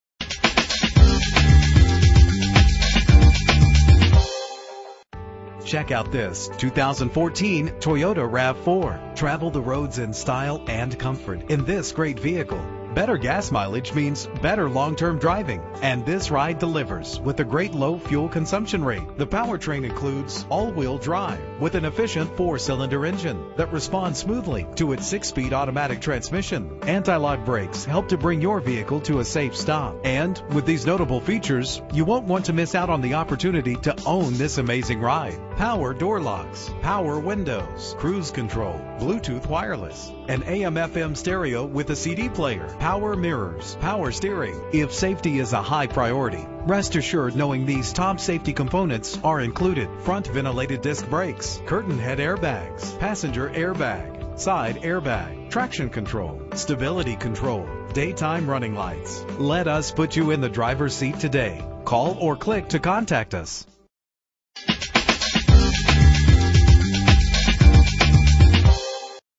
2014 Toyota RAV4 - Brookpark OH
Year: 2014
Make: Toyota
Model: RAV4
Trim: XLE
Engine: 2.5 liter inline 4 cylinder 16 valve
Transmission: 6-Speed Automatic
Color: Barcelona Red
Mileage: 10
Address:
13775 Brookpark Road